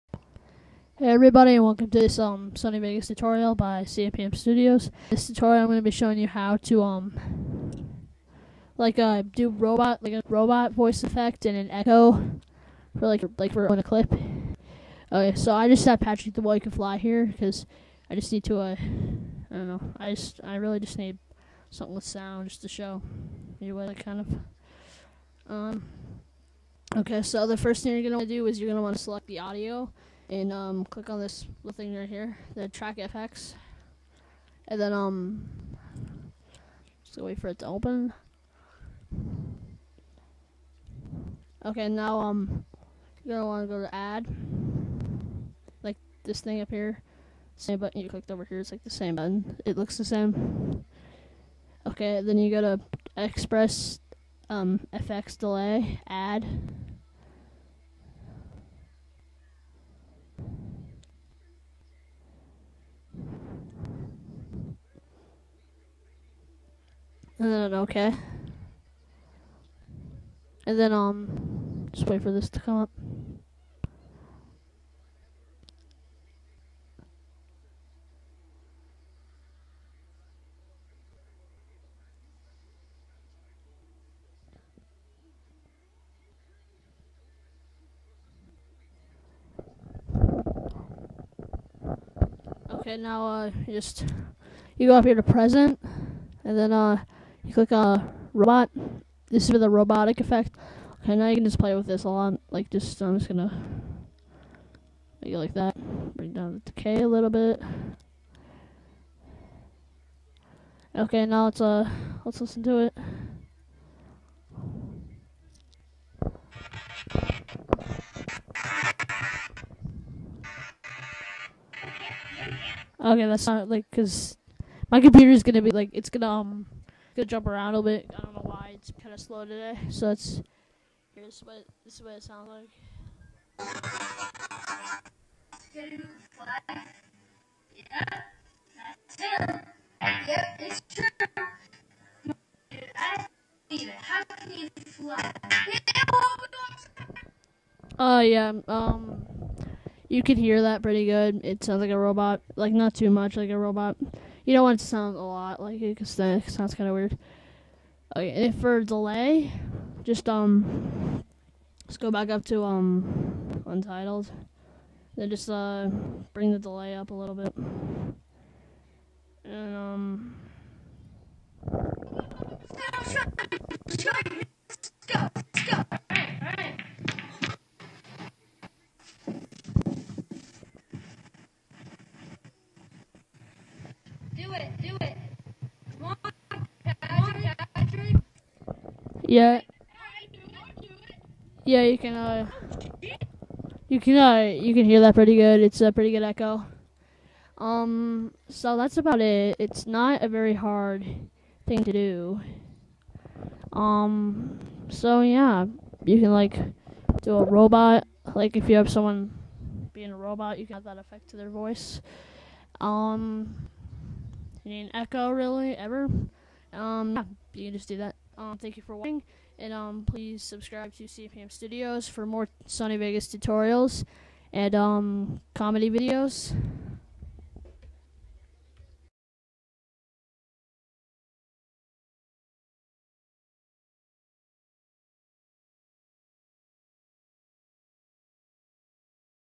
Robot Voice and Echo Effect in Sony Vegas
This is a Sony Vegas effect that will give your voice echo or make your voice sound like a robot's. It is an easy effect.